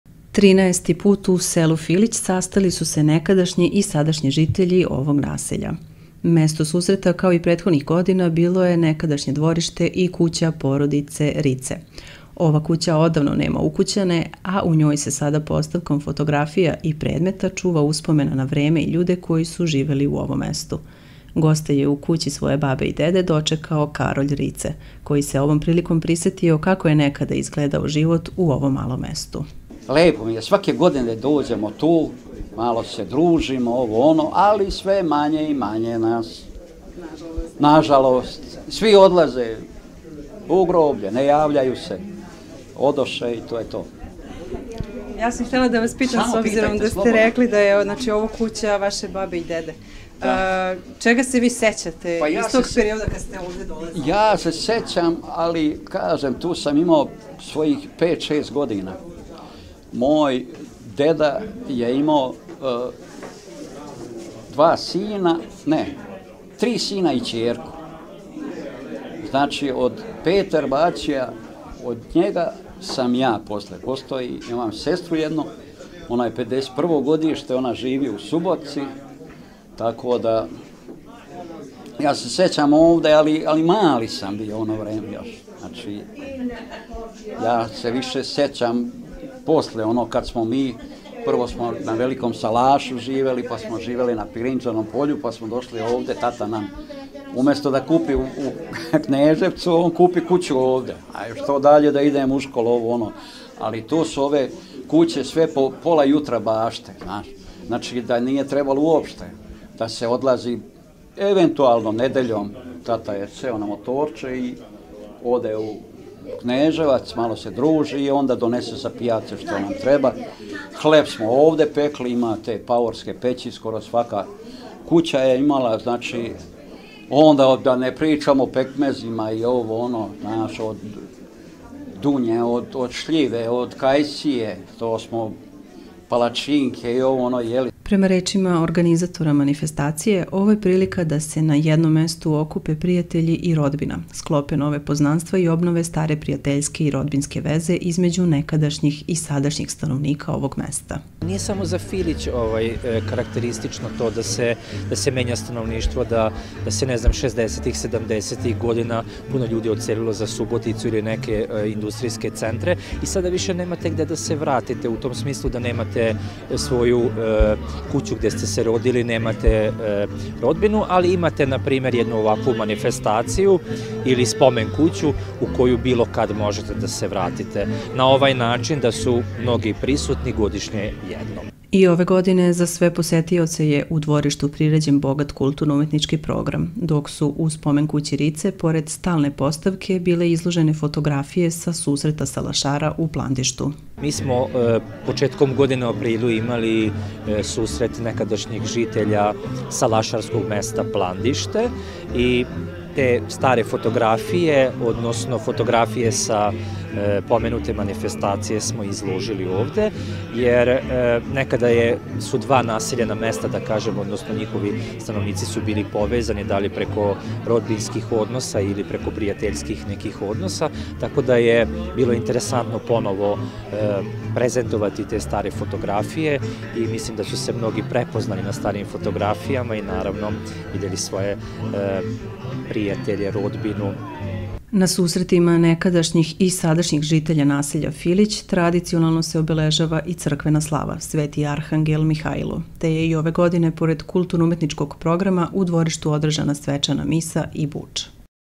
Susret nekadašnjih i sadašnjih žitelja sela Filić
Trinaesti put u selu Filić sastali su se nekadačnji i sadašnji žitelji ovog naselja. Mesto susreta kao i prethodnih godina, bilo je nekadašnje dvorište i kuća porodice Rice. Ova kuća odavno nema ukućane, a u njoj se sada postavkom fotografija i predmeta čuva uspomena na vreme i ljude koje su živeli u ovom mestu. Goste je u kući svoje babe i dede dočekao Karolj Rice, koji se ovom prilikom prisetio kako je nakada izgledao život u ovom malom mestu.
   Prema rečima organizatora manifestacije, ovo je prilika da se na jednom mestu okupe prijatelji i rodbina, sklope nove poznanstva i obnove stare prijateljske i rodbinske veze između nekadašnjih i sadašnjih stanovnika ovog mesta.
   I ove godine za sve posetioce je u dvorištu priređen bogat kulturno-umetnički program, dok su u spomen kući Rice pored stalne postavke, bile izložene fotografije sa susreta salašara u Plandištu.
   Na „Susretima nekadašnjih i sadašnjih žitelja naselja Filić“ tradicionalno se obeležava i crkvena slava – sveti arhangel Mihailo, te je i ove godine pored kulturno umetničkog-programa u dvorištu održana svečana misa i buč.